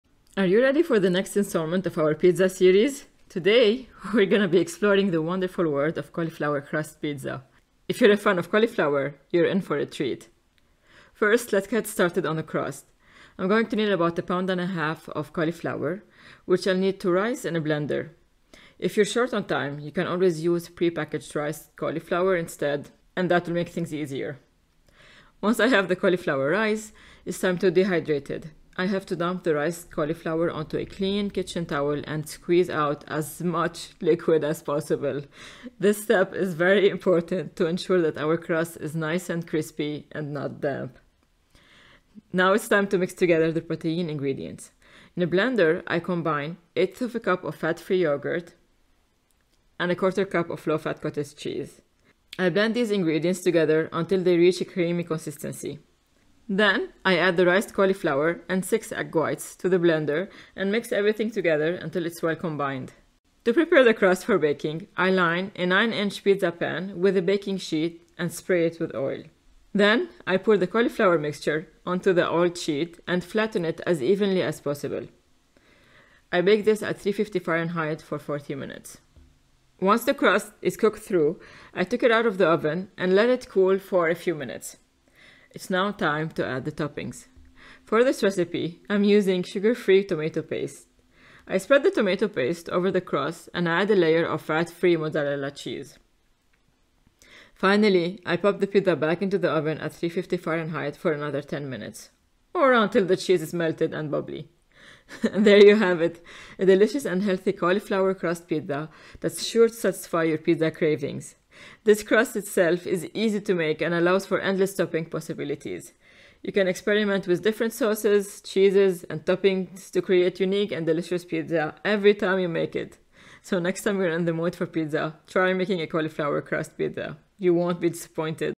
This 4 ingredients Cauliflower Crust Keto Pizza Recipe Will Blow Your Mind!
and then there was cauliflower!
Recipe:
- 1.5 lb cauliflower riced and squeezed
- 1/8 cup fat free yogurt
- 1/4 cup low fat cottage cheese
- 6 egg whites (12 tbs)

Bake at 350F 40 min. Add toppings of choice and Bake at 350F for another 10 min

Macros per slice:
33.7 cal, 0g Fat, 3.425g Carb, 4.8g Protein, 2.5mg Cholesterol